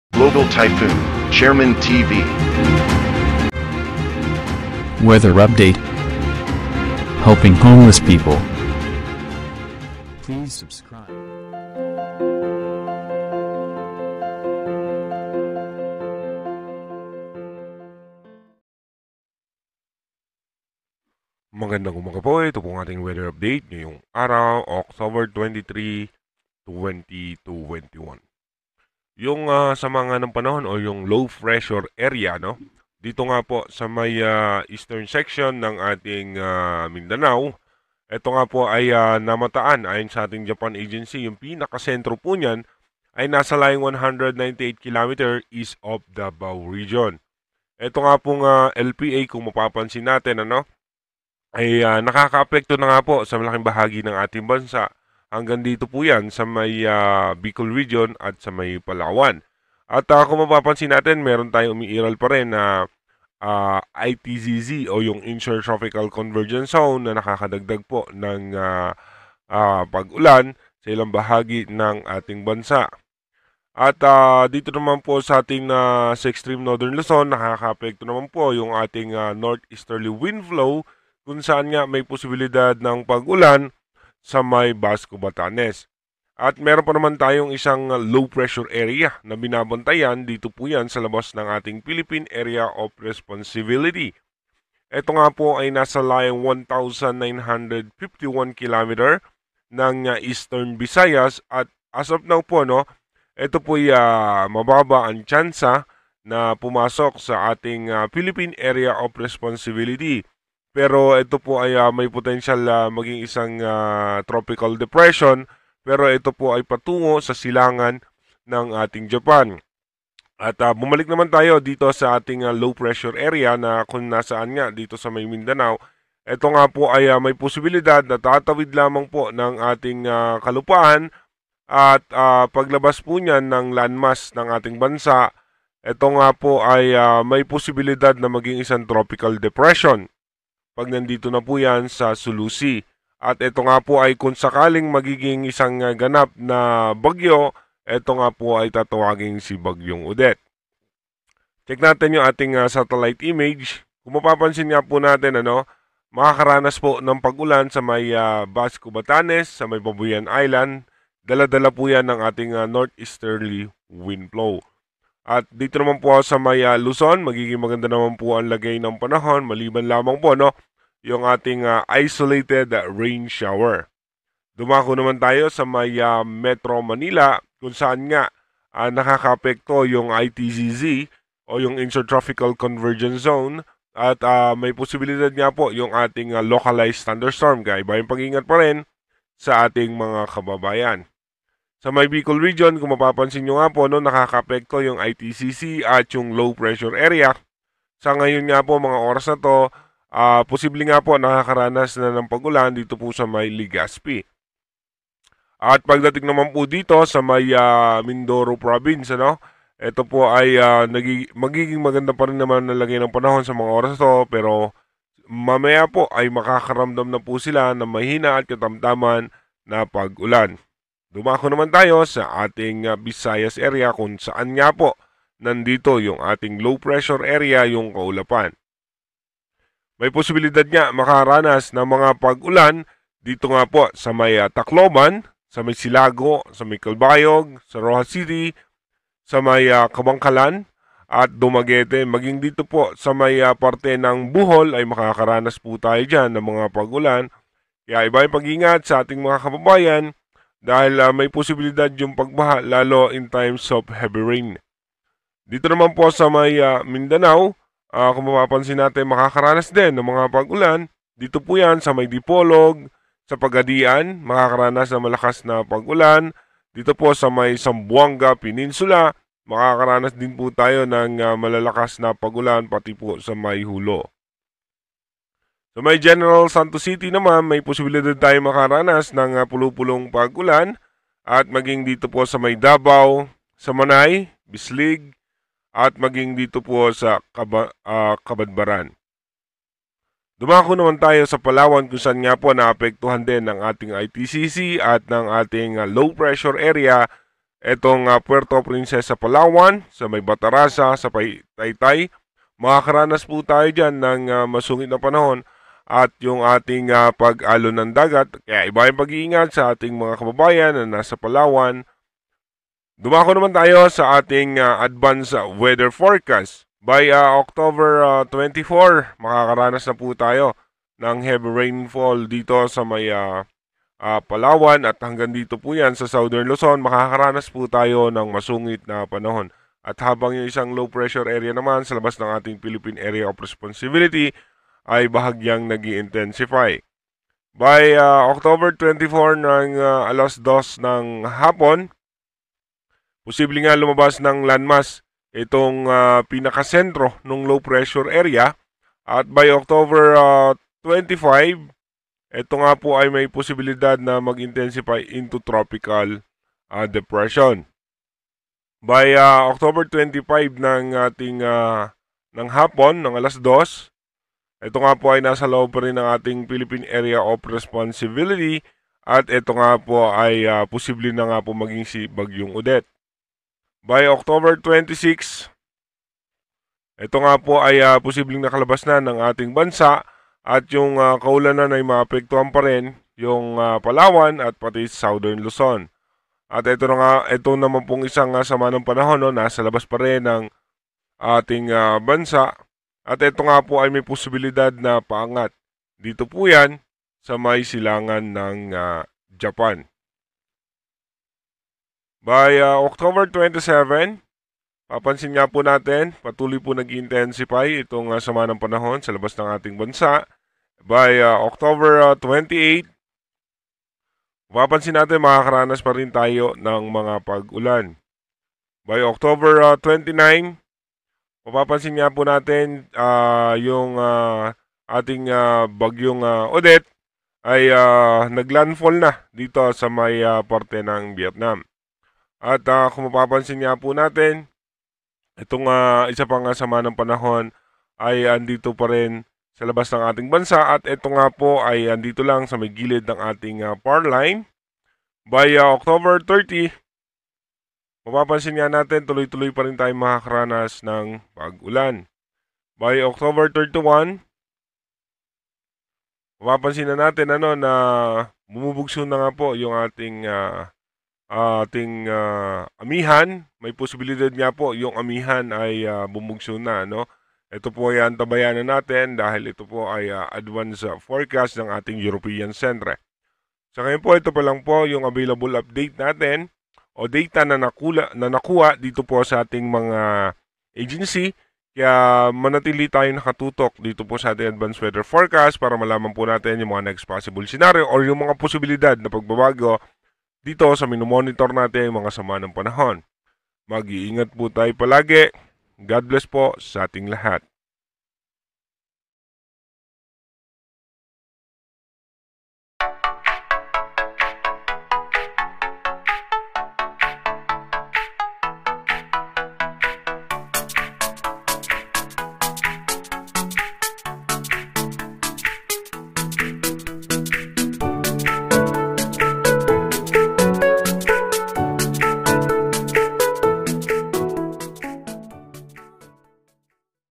POSIBLENG BAGYONG "ODETTE"?? ALAMIN DALAWANG LPA SA LOOB AT LABAS NG BANSA
SYNOPSIS: At 3:00 AM today, the Low Pressure Area (LPA) was estimated based on all available data at 75 km Southeast of Tacloban City, Leyte (10.7°N, 125.4°E) embedded along the Intertropical Convergence Zone (ITCZ) affecting Palawan, Visayas, and Mindanao. Meanwhile, another Low Pressure Area (LPA) was estimated based on all available data at 198 km east of davao (8.3°N, 122.5°E).

Visayas, Zamboanga Peninsula, BARMM, and Palawan Cloudy skies with scattered rainshowers and thunderstorms ITCZ / LPA Possible flash floods or landslides during severe thunderstorms

Batanes and Babuyan Islands Cloudy skies with light rains Northeasterly surface windflow No Significant impact

Metro Manila and the rest of the country Partly cloudy to cloudy skies with isolated rainshowers or thunderstorms ITCZ / Localized thunderstorms Possible flash floods or landslides during severe thunderstorms

DOST-PAGASA SITE
WINDY APPS
ZOOM EARTH